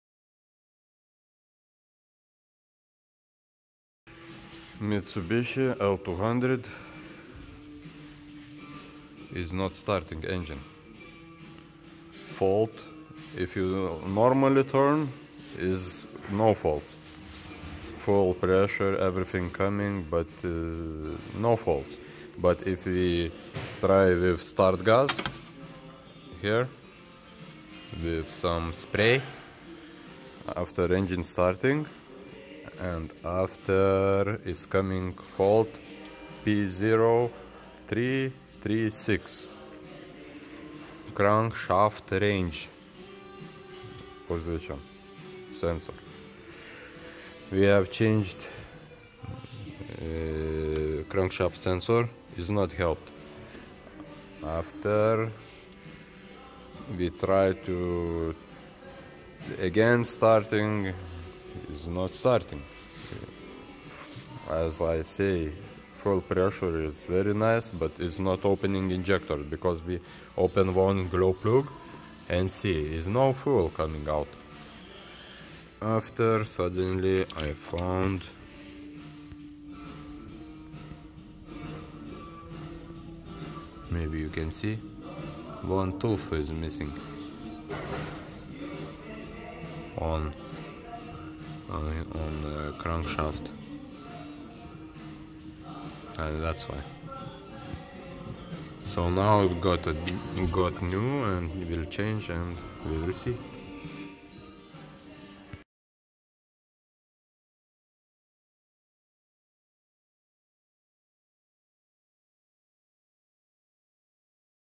Mitsubishi P0336 crank angle sensor range
Mitsubishi not starting  fault P0336 Crank angle sensor range
fault P0336 Crank angle sensor range